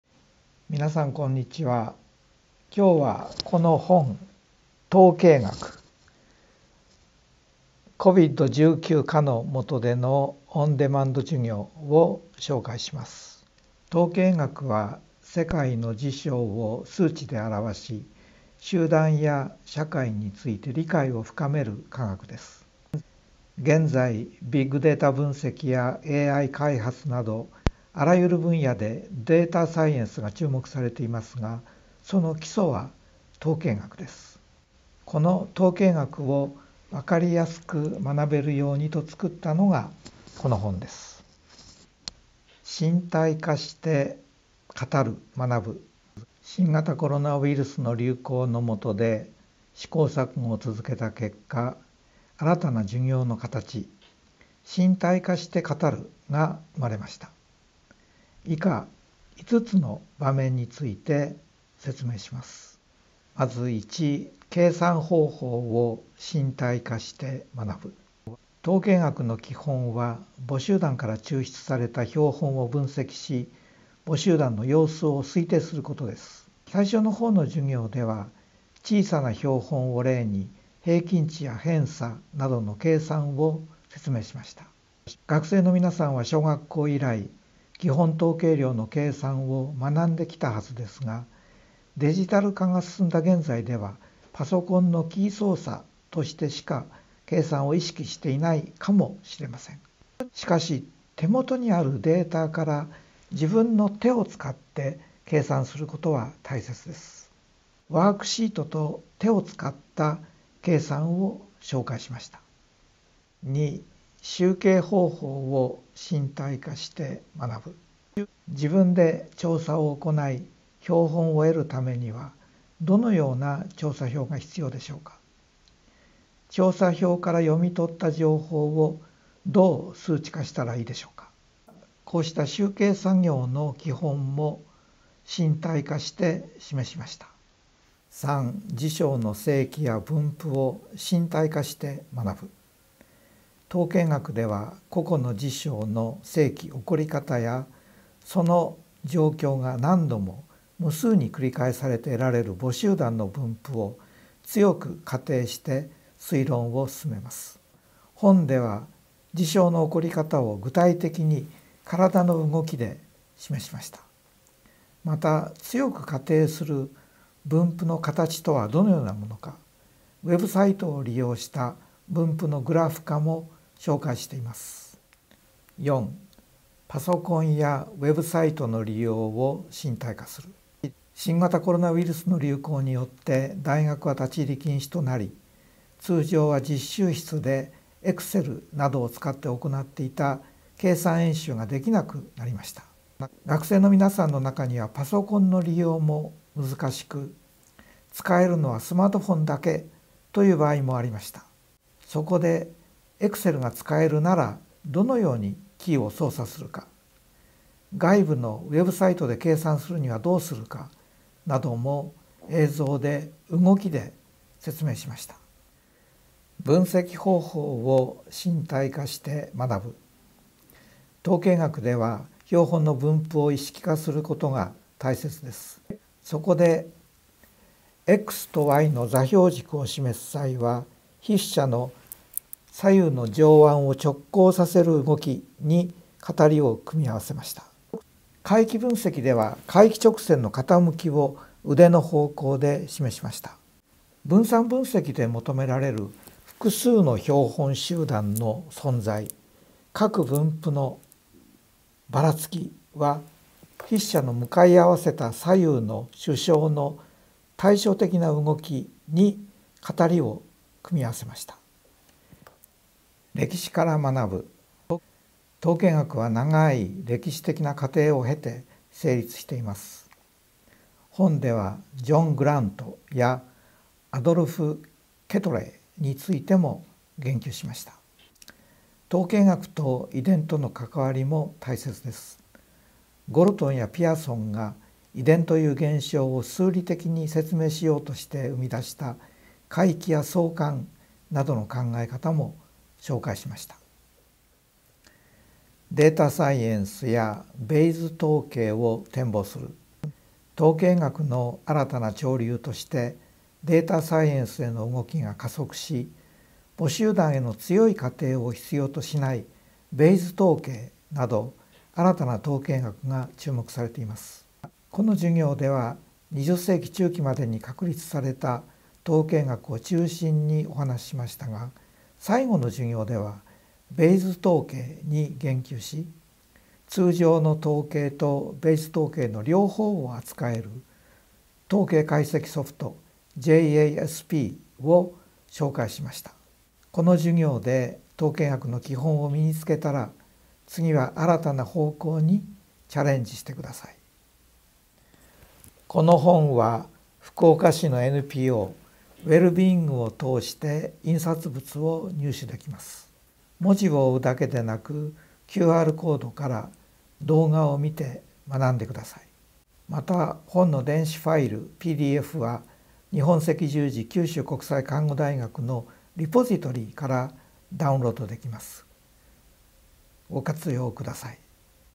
身体化して学ぶ統計学；本のご紹介
・統計学とは；　世界の事象を数値で表わし、集団や社会について理解を深める科学です。現在、ビッグデータ分析やAI開発など広い分野でデータサイエンスが注目されていますが、基礎は統計学です。統計学を分かりやすく学べるようにと作ったのがこの本です。
・身体化して語る／学ぶ
　COVID-19禍のもとで試行錯誤を続けた結果、新たな授業の形「身体化して語る／学ぶ」が生まれました。本の特徴は以下の５点です。
１）計算方法の身体化；　統計学の基本は、母集団から抽出された標本を分析し、母集団の様子を推定することです。最初の方の授業では小さな標本を例に、平均値や偏差などの計算を説明しました。皆さんは、小学校以来、基本統計量の計算を学んで来たはずですが、デジタル化が進んだ現在ではパソコンのキー操作としてしか計算を意識していないかもしれません。しかし手元にあるデータから自分の手を使って計算することは大切です。ワークシートと手を使った計算を紹介しました。
２）集計方法の身体化；　調査を行い標本を得るためには、どのような調査票が必要でしょうか。調査票から読み取った情報をどう数値化したらよいでしょうか。こうした集計の基本も身体化して示しました。
３）事象の生起や分布の身体化；　統計学では、個々の事象の生起や、その生起・状況が何度も（無数に）繰り返されて得られる母集団の分布を強く仮定して推論を進めます。本では事象の起こり方を体の動きで示しました。また強く仮定する分布の形とはどのようなものか、ウェブサイトを利用した分布のグラフ化も紹介しています。
４）パソコン/ウェブサイト利用の身体化；　COVID-19禍で大学は立入禁止になり、通常は実習室でエクセルなどを使って行っていた計算演習が出来なくなりました。学生の中にはパソコンの利用も難しく、使えるのはスマートフォンだけ、という場合もありました。そこでエクセルが使えるならどのようにキーを操作するか、外部のウェブサイトで計算するにはどうするか、なども映像で、動きで説明しました。
５）分析方法の身体化；　統計学では標本の分布を意識化することが大切です。XYの座標軸を示す際は「筆者の左右の上腕を直交させる動き」に語りを組み合わせました。回帰分析では回帰直線の傾きを腕の方向で示しました。分散分析で求められる「複数の標本集団の存在、各分布とばらつき」は「筆者の向かい合わせた左右の手掌の対称的な動き」に語りを組み合わせました。
・歴史から学ぶ；　統計学は長い歴史的な過程を経て成立しています。本ではグラウントやケトレーについても言及しました。統計学と遺伝との関わりも大切です。ゴルトンやピアソンが遺伝という現象を数理的に説明しようとして生み出した回帰や相関などの考え方も紹介しました。
・データサイエンス＆ベイズ統計の展望；　統計学の新たな潮流として、データサイエンスへの動きが加速し、母集団への強い仮定を必要としないベイズ統計など新たな統計学が注目されています。授業では20世紀中期までに確立された統計学を中心にお話ししましたが、最後はベイズ統計に言及し、通常統計とベイズ統計の双方を扱える統計解析ソフトJASPを紹介しました。本で統計学の基本を身につけたら、次は新たな方向にチャレンジしてください。
・入手方法：本はNPOウェルビーイングから入手できます。文字を読み、QRコードから動画を見て学んでください。
・本の電子ファイルPDFは日本赤十字九州国際看護大学のリポジトリからダウンロードできます。